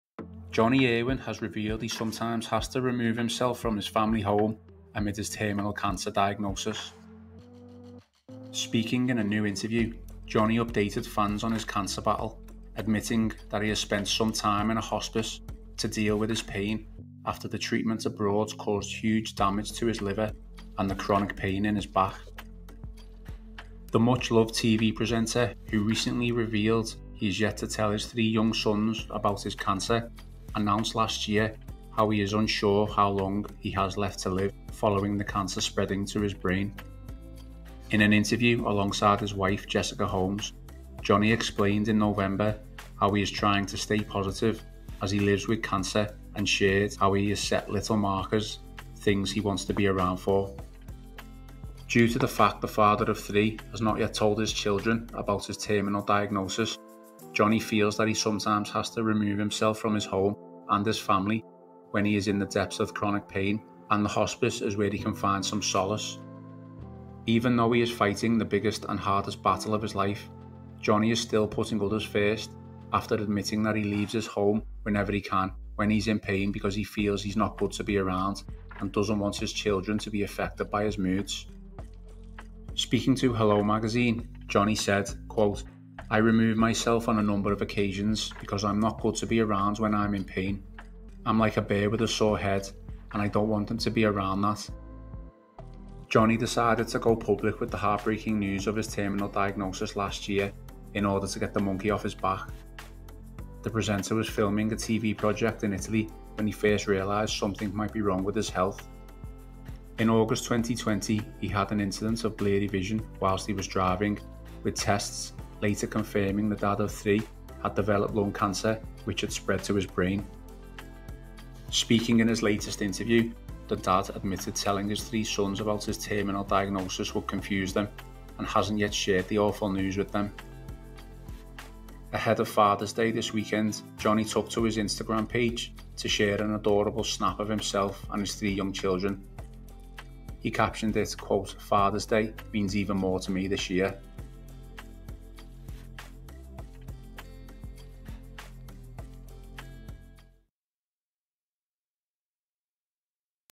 Jonnie Irwin 'removes himself' from family home to stay in hospice amid terminal cancer
Former A Place In The Sun presenter Jonnie Irwin sometimes has to 'remove himself' from his home and his family when he is in the depths of chronic pain - and the hospice is where he can find some solace